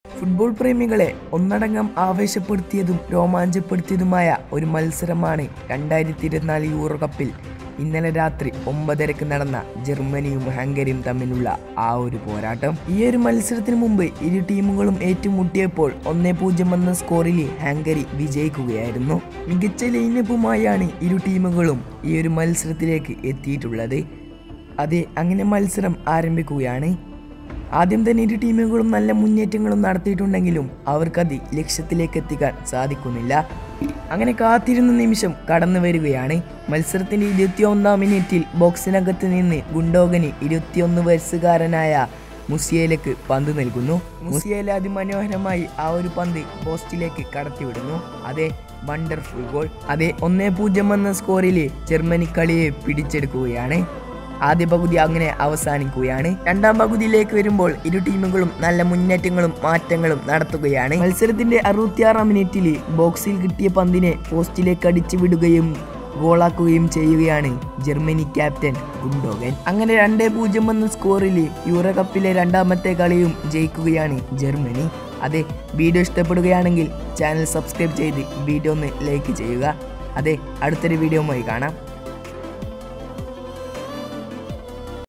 യൂറോപ്പിൽ അടിച്ചു 👊🏻 കയറി ജർമ്മനി🔥 | Match Review Malayalam | 2024 Euro cup
Match Review Malayalam | 2024 Euro cup
Germany win
musiala goal